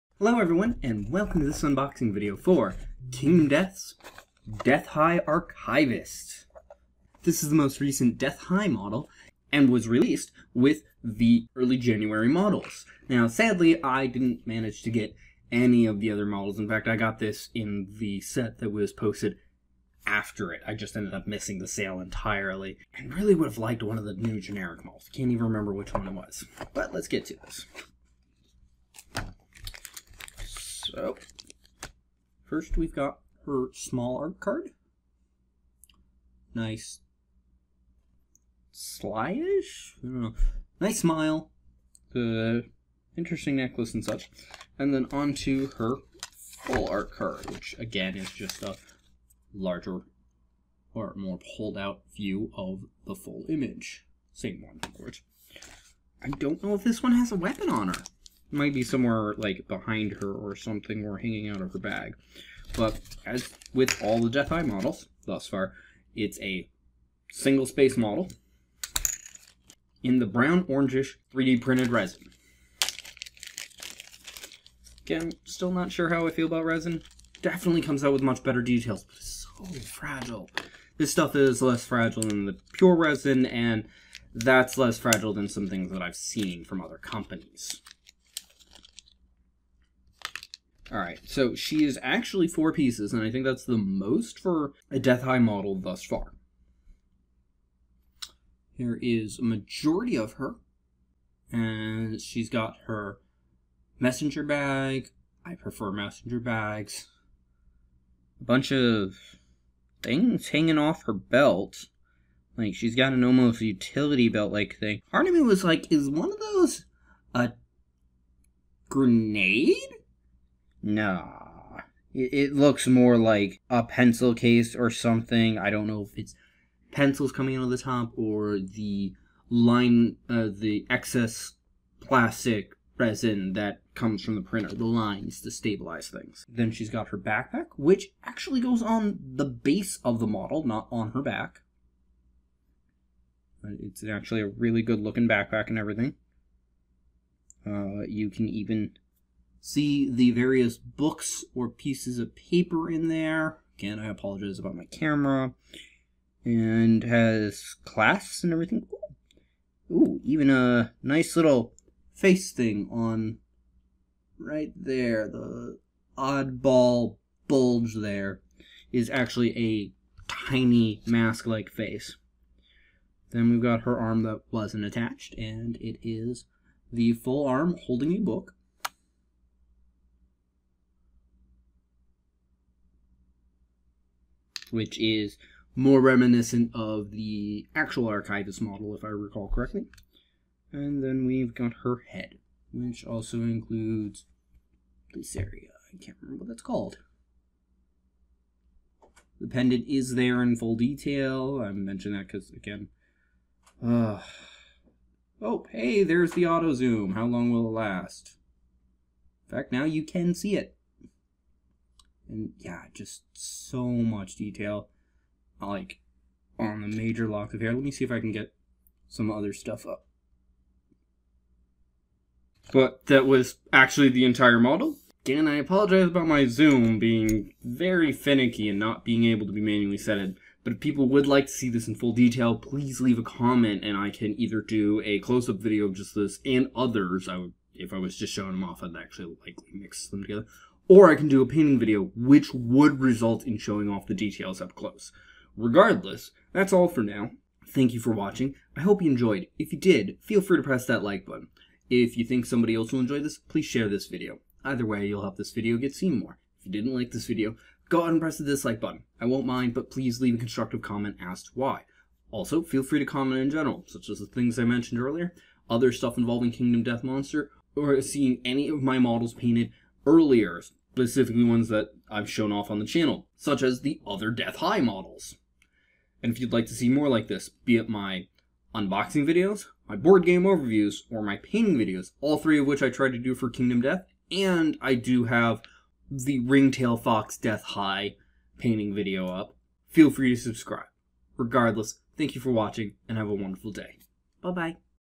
Unboxing Kingdom Death Death High Archivist
This is an unboxing video of the Death High Archivist from Kingdom Death.